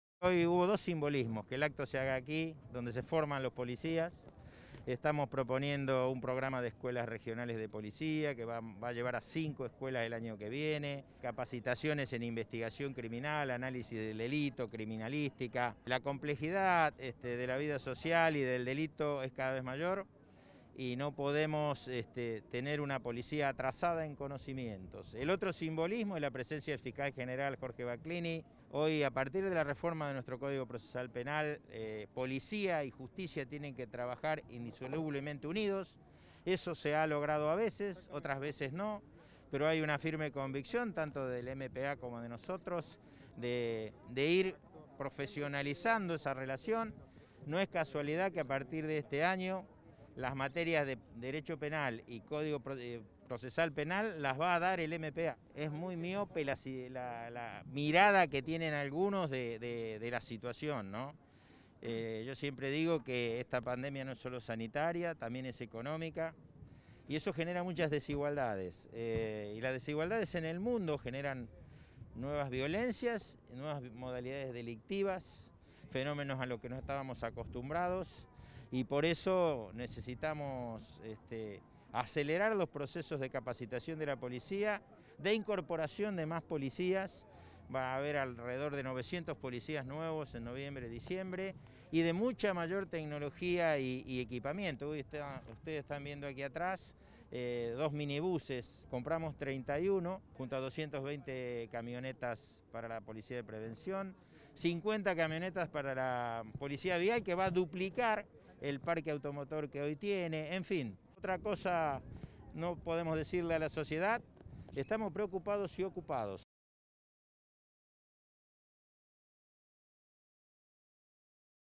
LA POLICÍA DE LA PROVINCIA DE SANTA FE CELEBRÓ SU 157 ANIVERSARIO
El ministro de Seguridad, Jorge Lagna, encabezó el acto realizado en el ISeP.